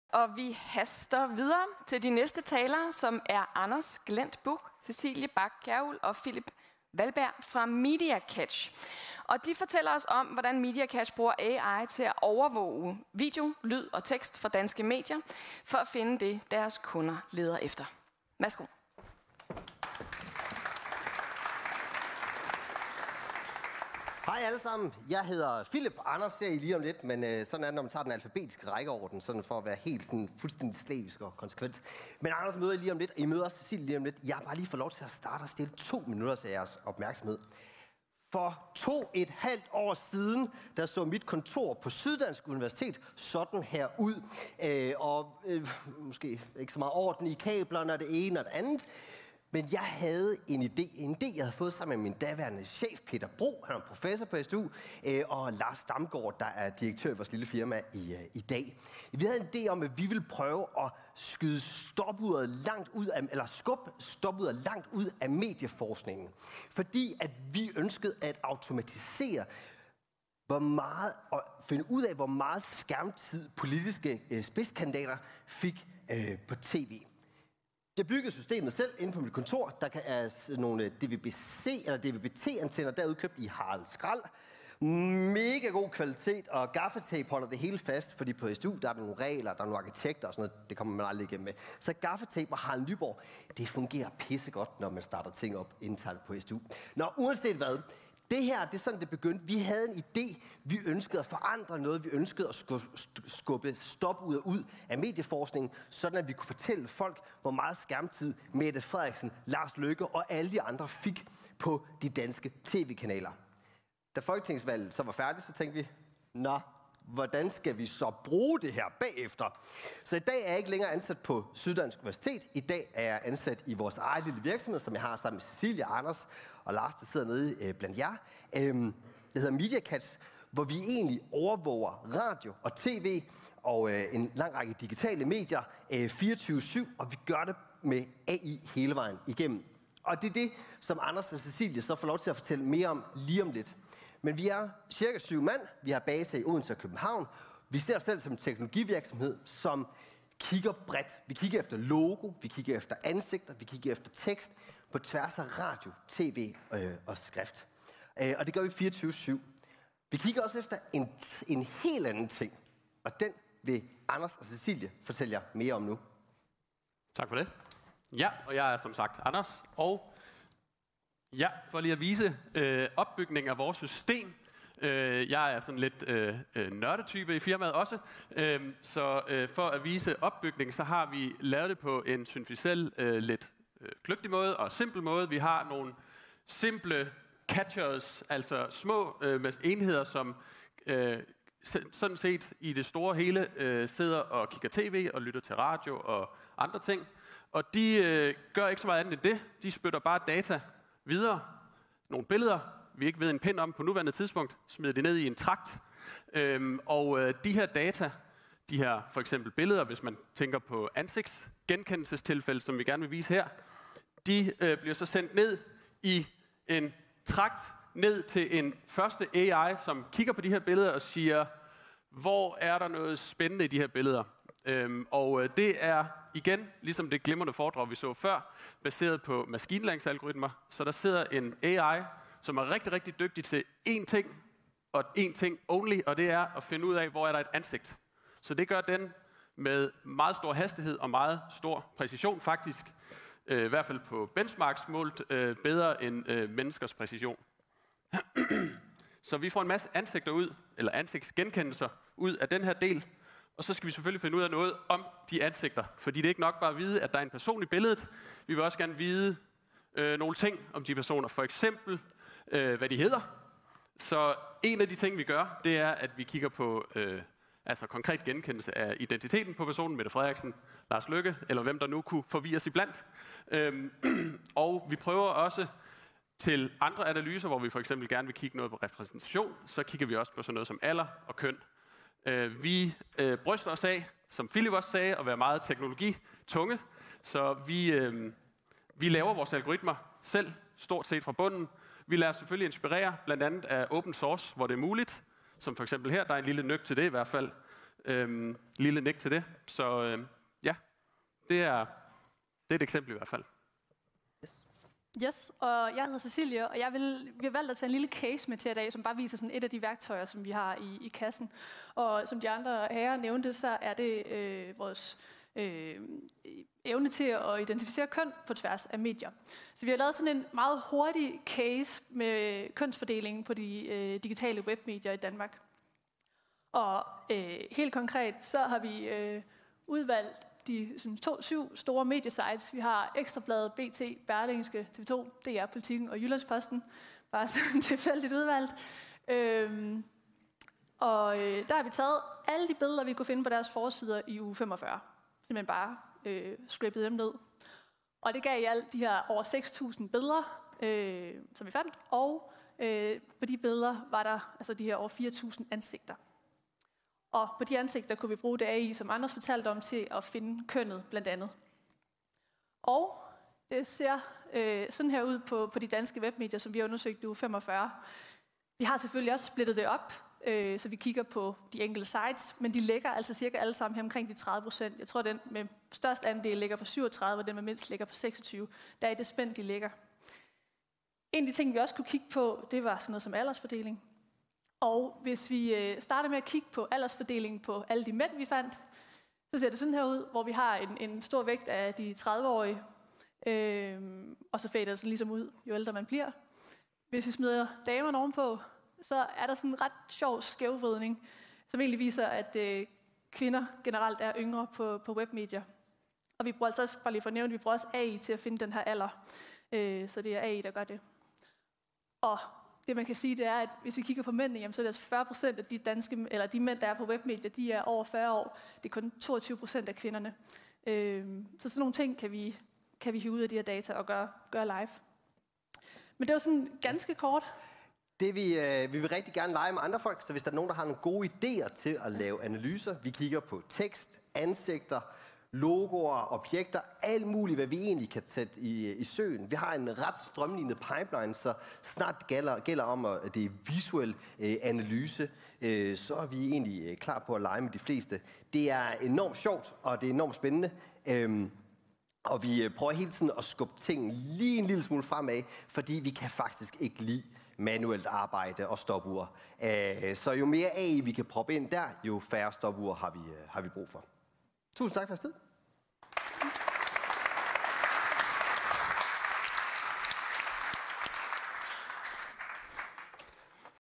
MediaCatch: Sådan bruger vi AI til at skabe next gen medieovervågning
CAIO Anders Glent Buch, CDO Cæcilie Bach Kjærulf og CTO Filip Wallberg fortæller om, hvordan MediaCatch anvender computer vision og andre AI teknologier til at identificere personer, køn, alder og logoer mv. i nyhedsvideoer og nyhedsbilleder